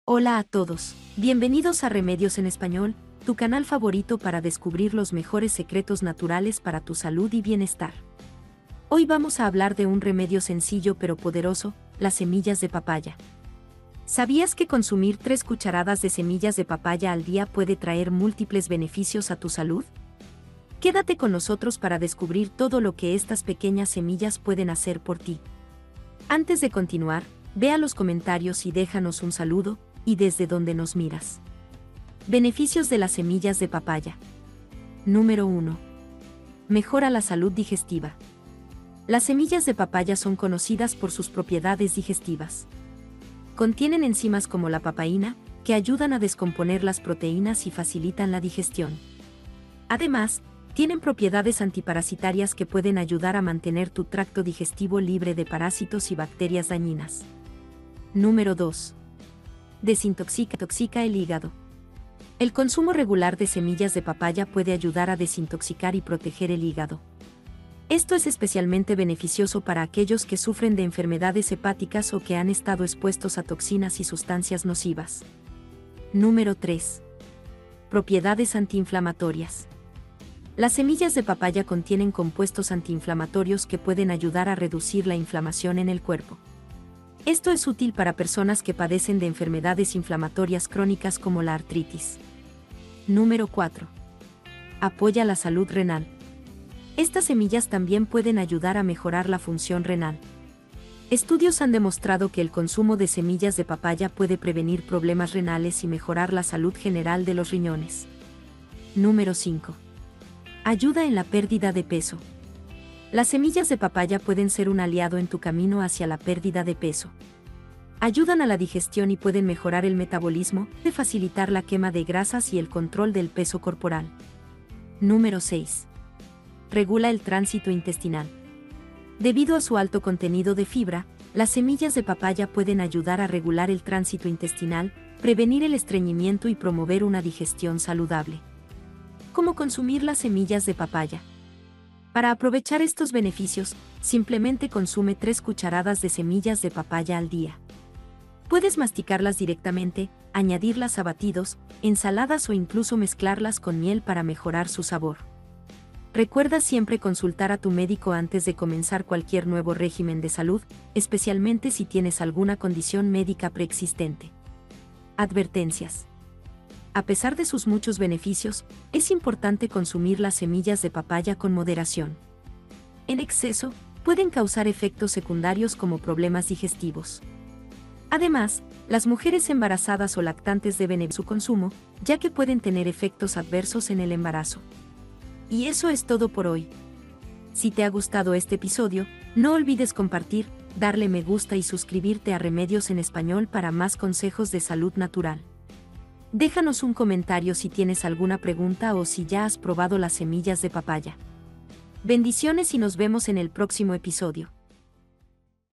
Hasta Desintoxica el Hígado! 3 Cucharadas de Semillas de Papaya o Lechosa y esto Pasará en tu Cuerpo
En este video de Remedios en Español, te mostramos los increíbles beneficios de consumir 3 cucharadas de semillas de papaya o lechosa al día. Desde mejorar la digestión hasta desintoxicar el hígado, estas pequeñas semillas son un tesoro de salud natural. Aprende cómo incorporarlas en tu dieta diaria y las advertencias que debes tener en cuenta. ¡No te pierdas esta valiosa información para mejorar tu bienestar!